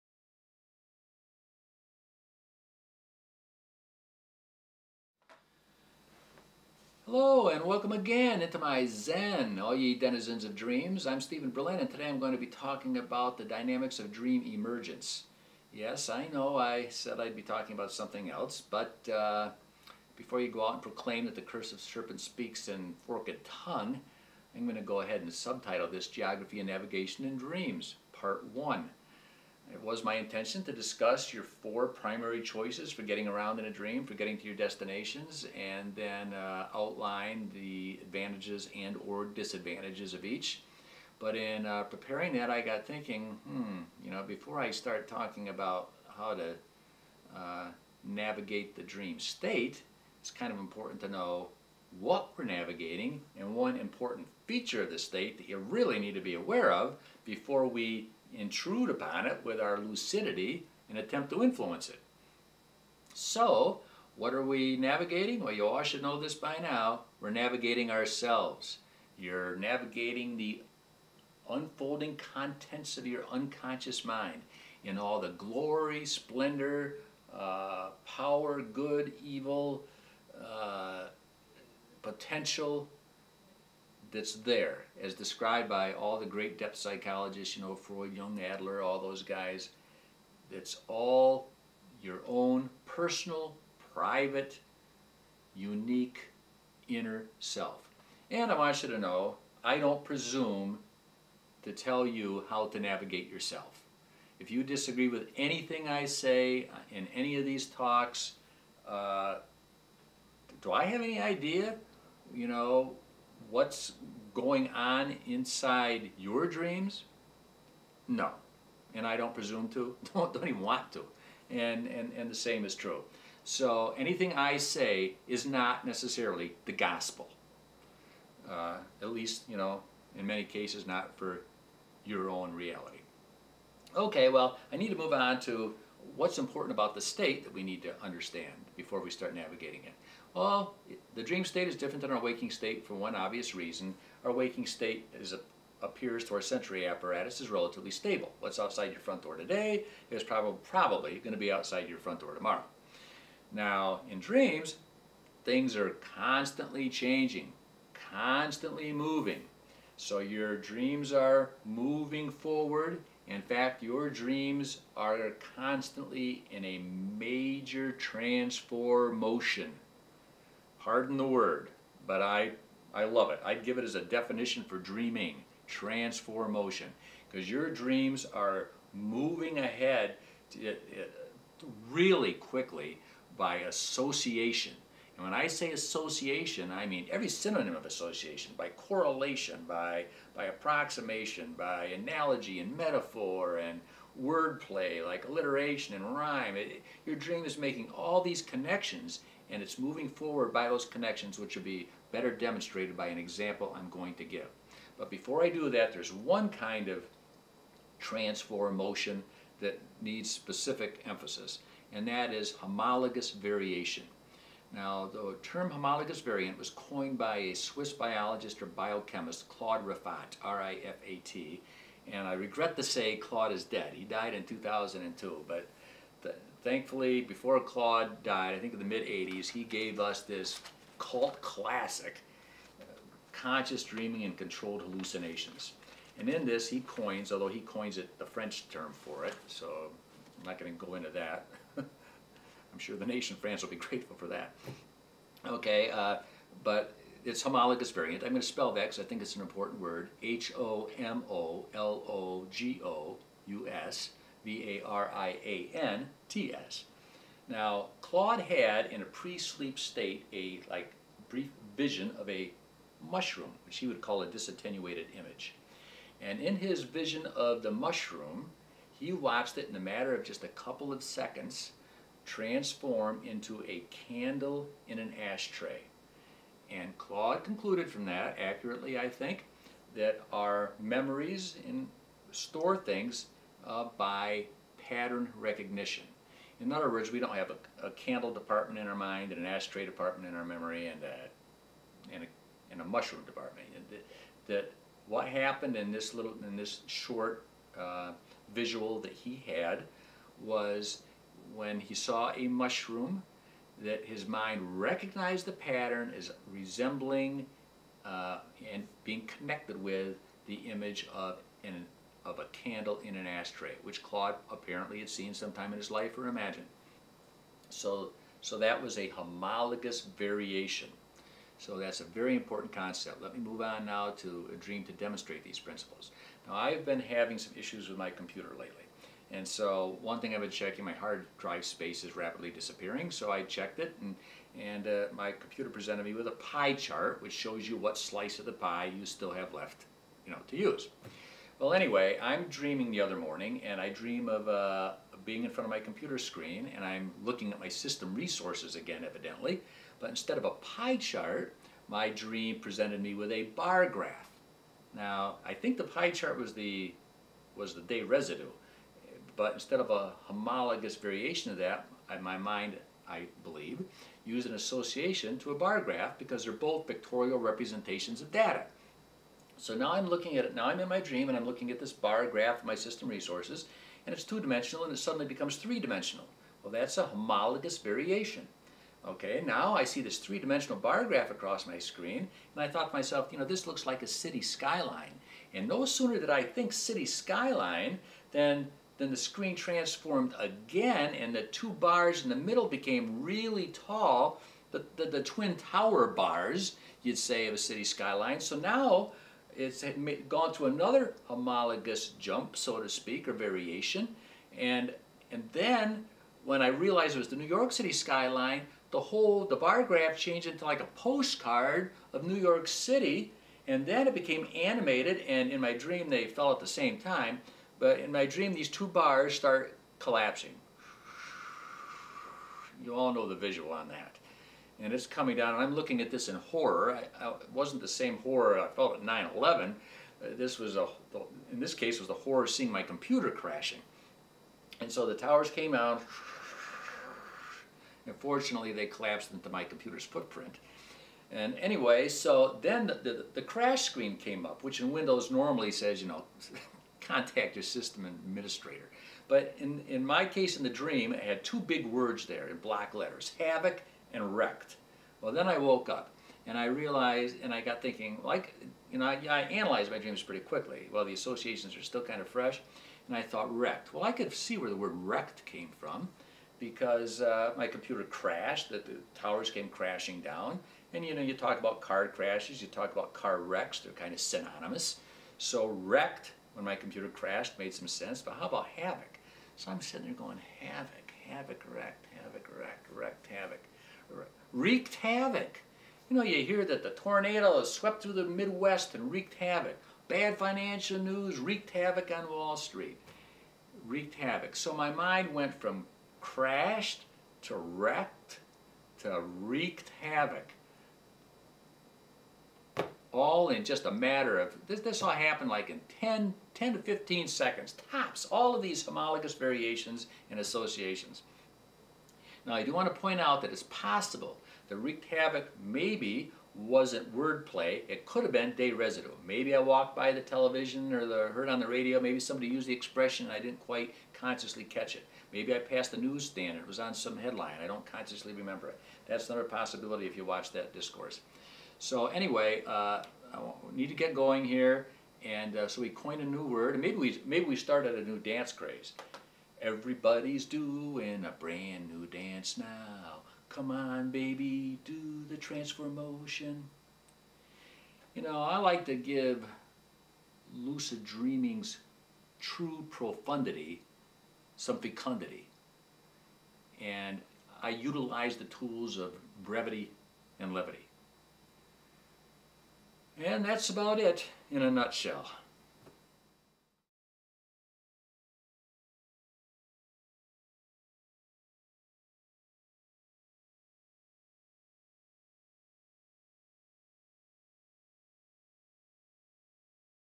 Lucid Dreaming #9 - Dynamics of Dream Emergence - (Original 2007 Series)
Topics in these presentations include Dreaming, Lucid Dreaming and Transcendent States of Consciousness accessible through your dreams. I don’t attribute paranormal, religious or other interpretations to my content. Originally released in 2007, their relevance and value hasn’t aged.